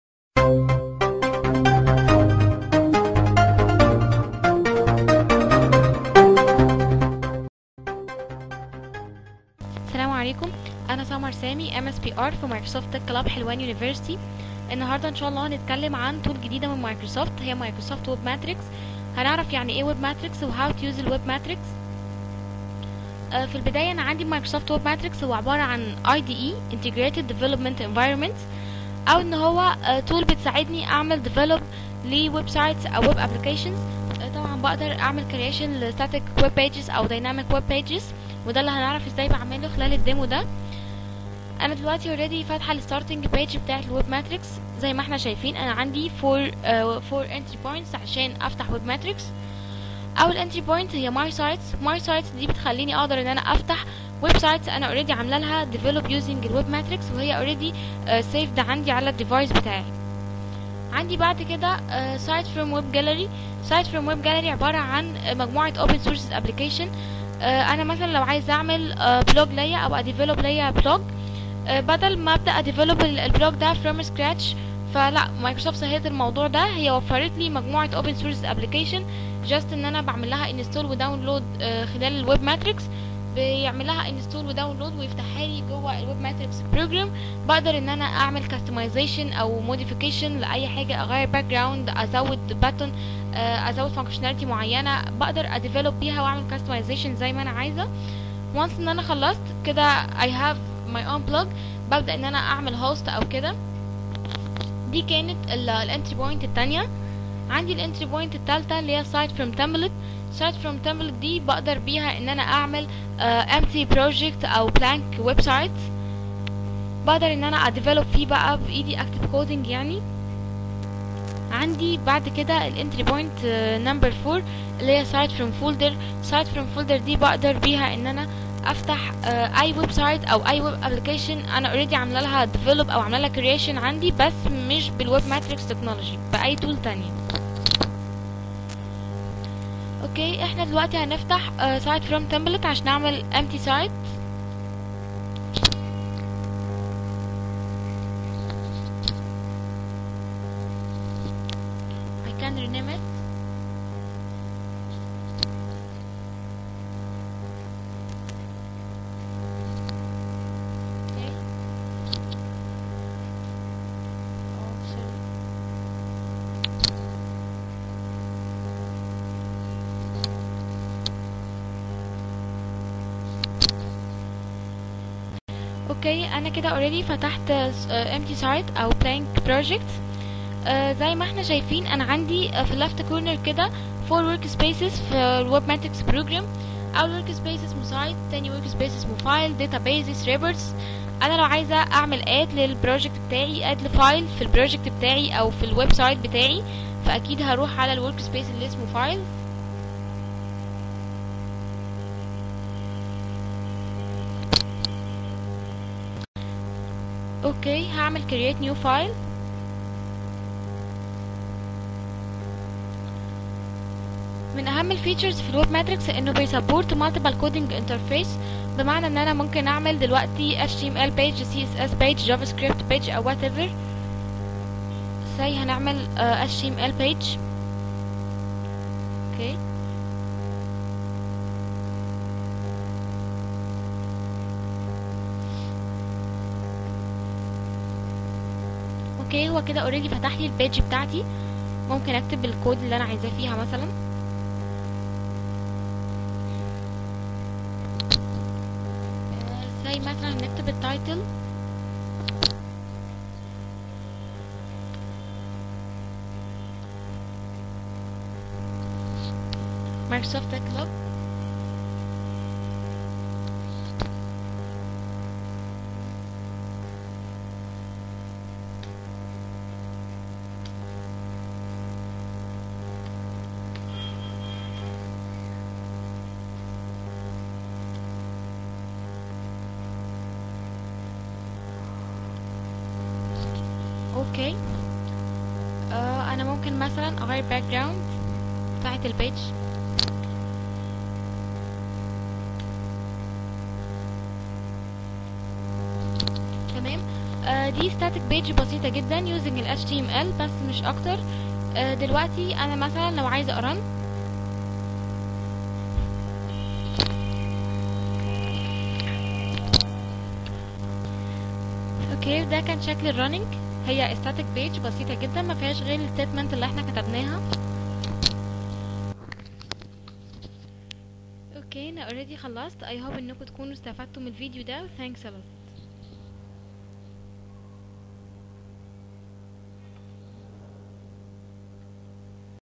microsoft web matrix .wmv
this video will help you learn how to take the first step into microsoft web matrix , what's web matrix and how to use it